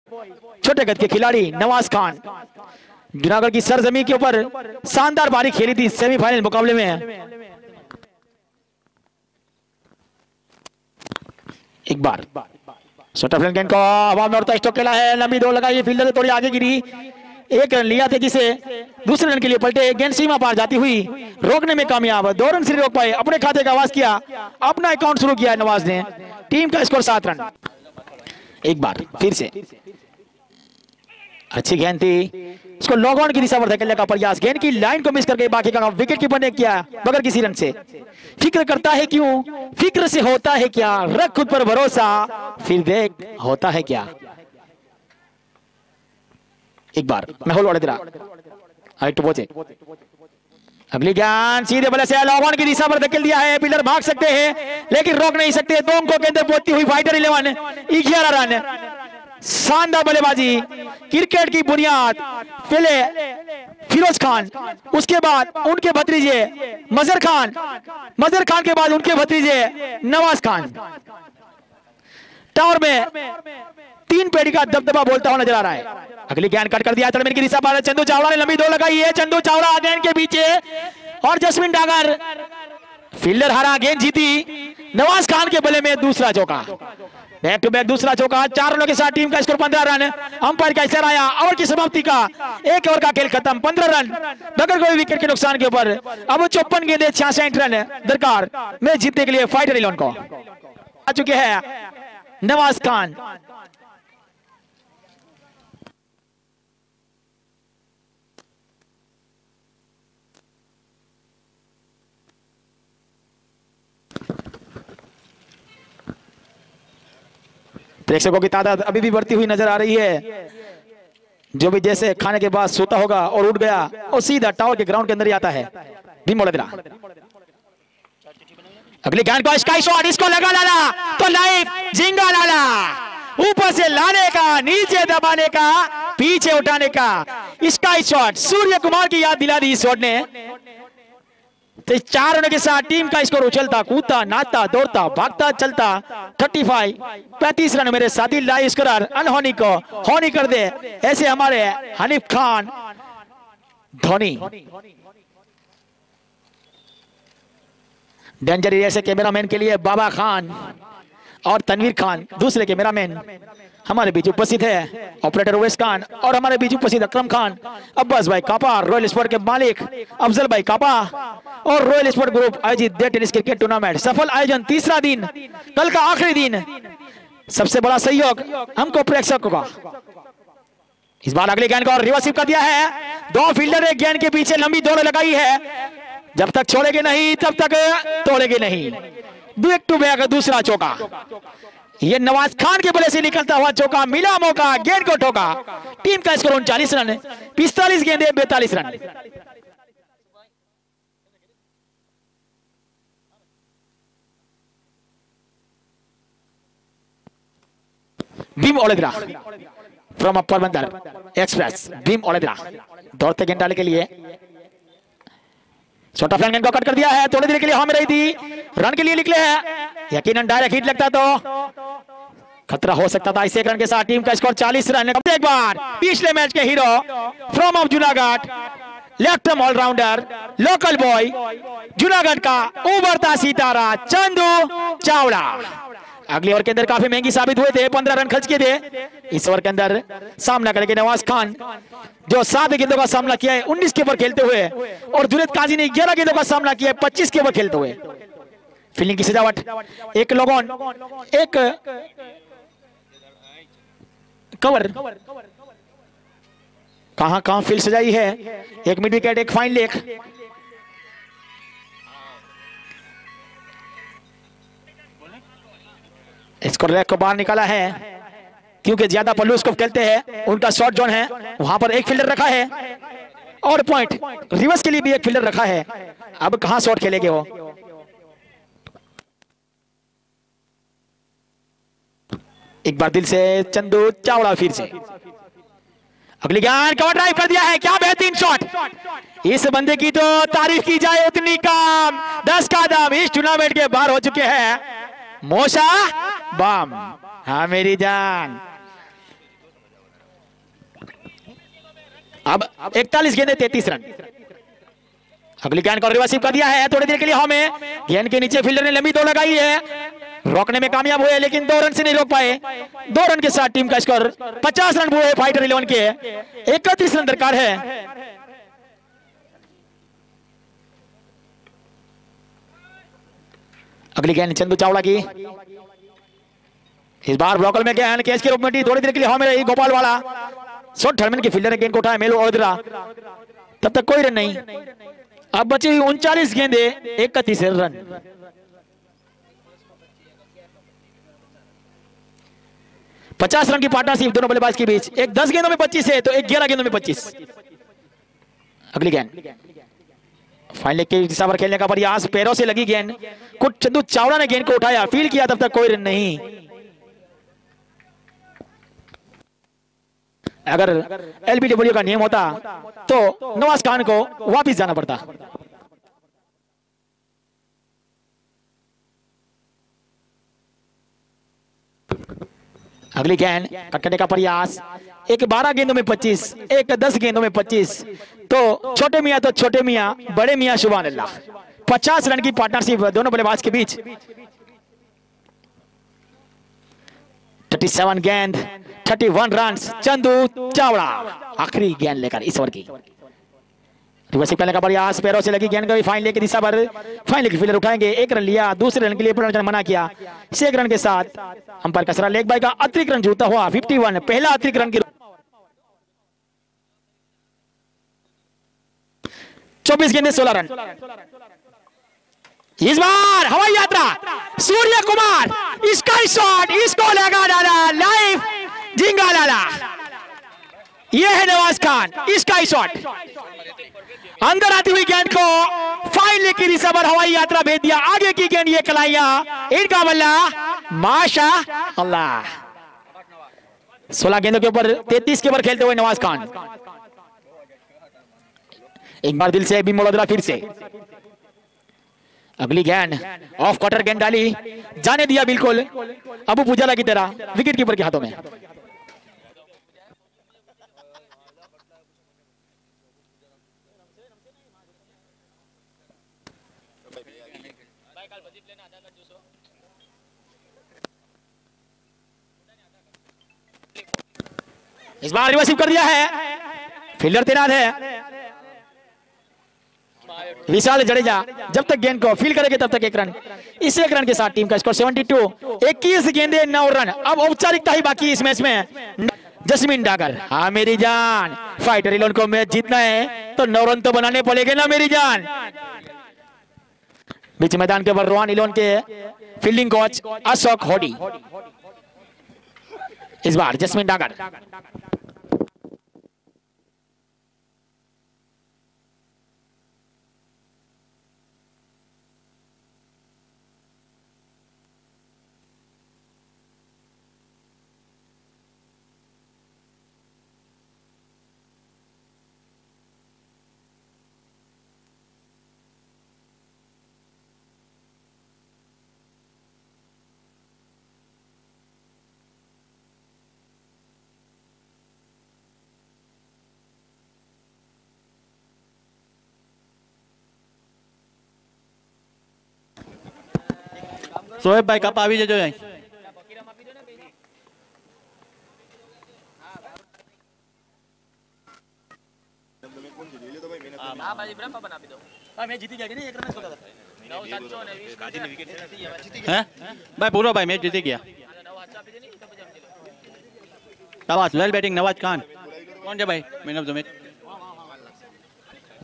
41 Runs in just 19 Ball - Nawaz khan || MPL - 2023 || UT Sports LIVE
41 Runs in just 19 Ball - Nawaz khan || MPL - 2023 || UT Sports LIVE

For More Live Sports Video

THIS VIDEO IS ONLY PROMOTION PURPOSE. SONGS PLAYED DURING EVENT ARE THE PROPERTY OF RESPECTED COMPINES.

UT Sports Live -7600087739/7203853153